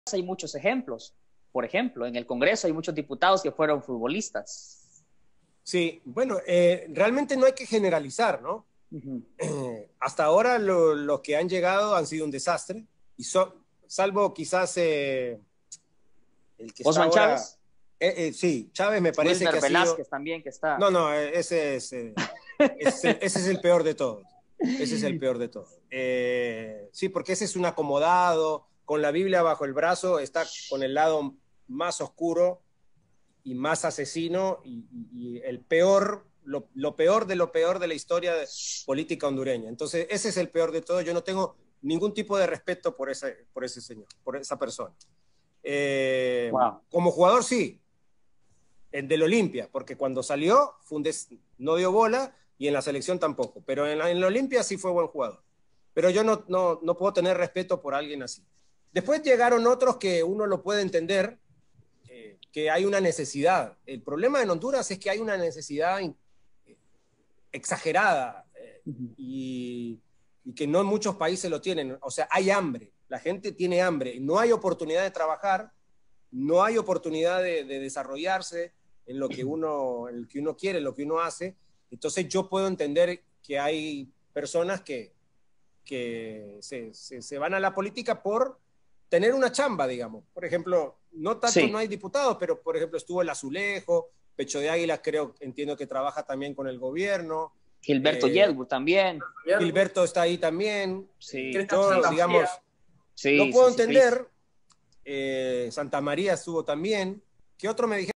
Copán Álvarez arremete contra Wilmer Velásquez
El periodista hondureño de Telemundo cuestionó al exfutbolista y hoy diputado.